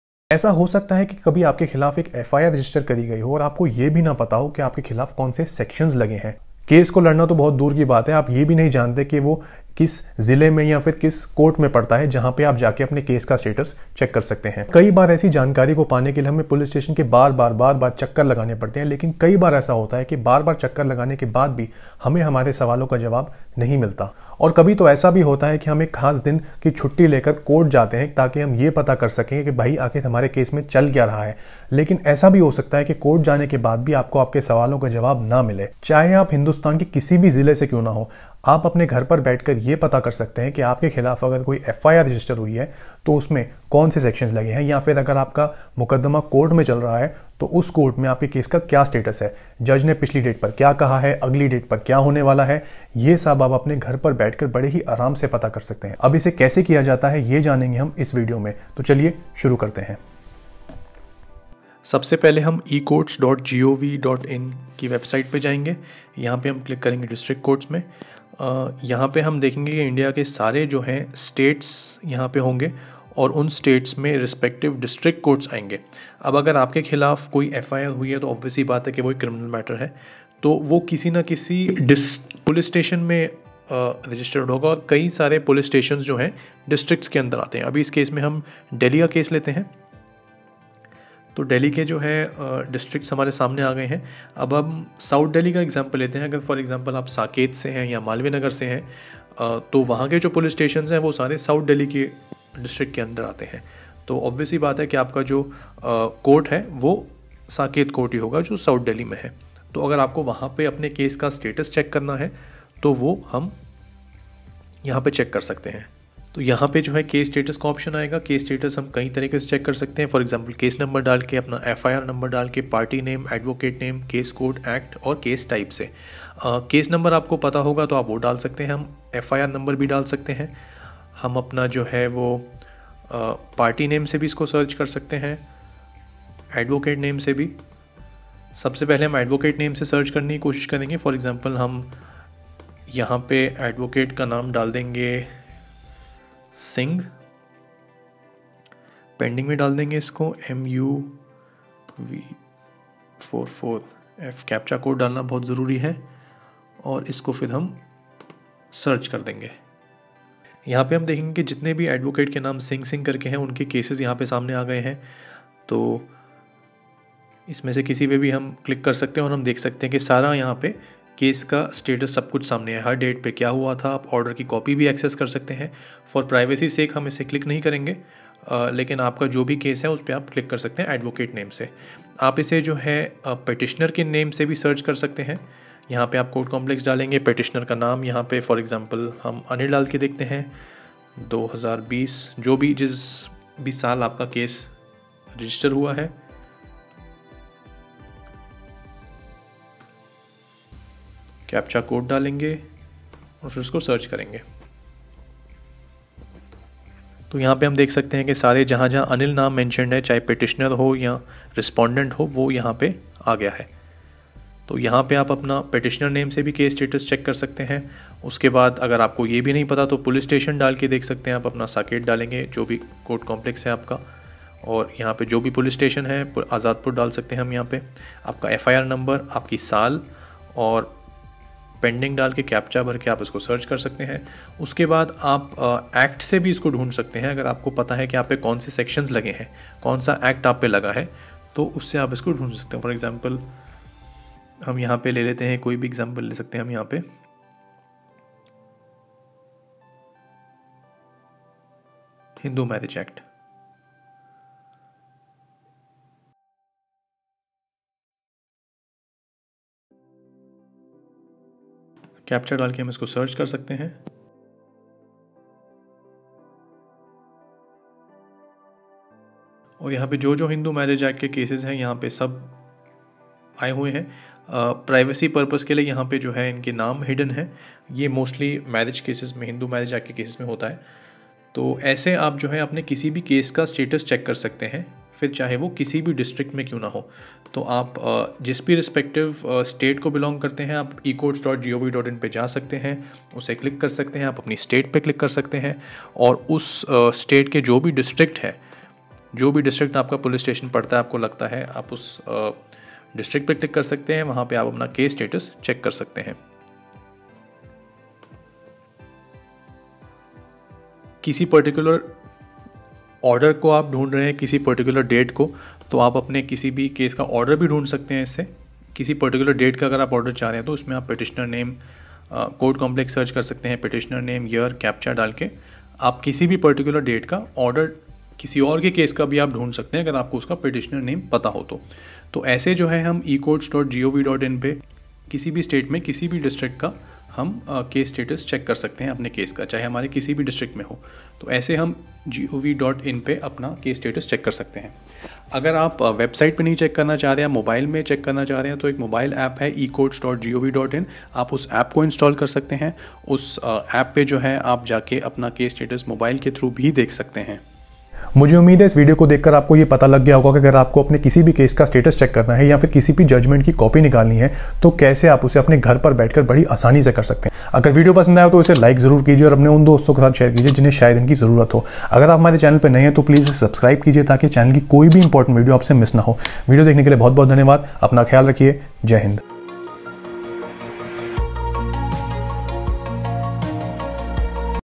How to Check Case Status ONLINE from home in 2021 | Check Case Status in any District court in India
You can check your case status in India .. Check status from home.. How to check ? Full information..